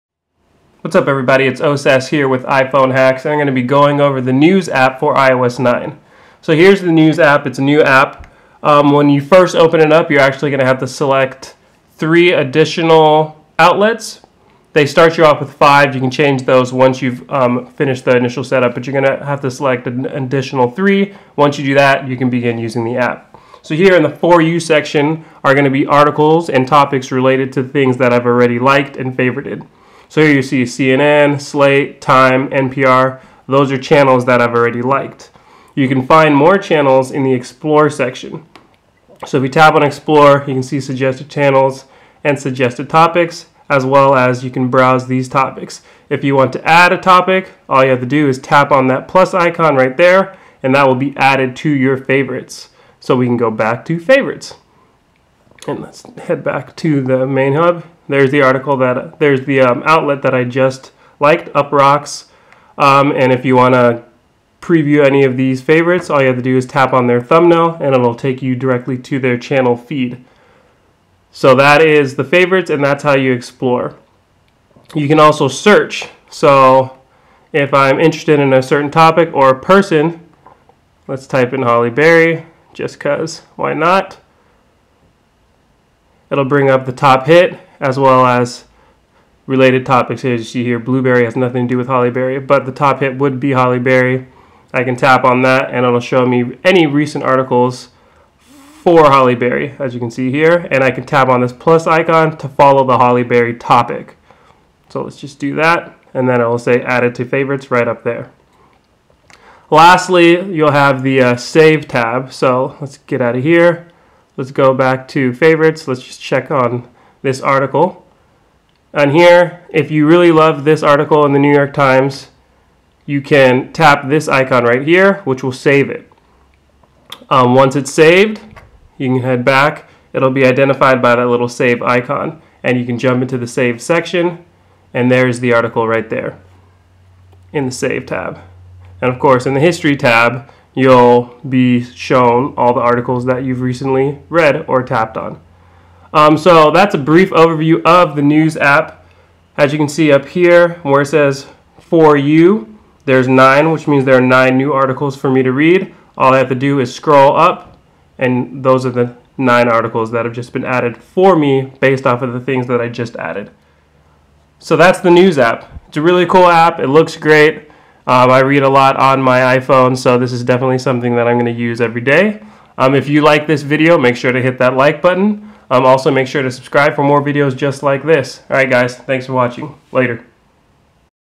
Getting to Know the iOS 9 News App - iPhone Hacks
Hands-on with iOS 9's News app. The News app is one of the standout applications of iOS 9. News curates articles based on your preferences and places them in a visually appealing magazine-style layout. Let's check it out!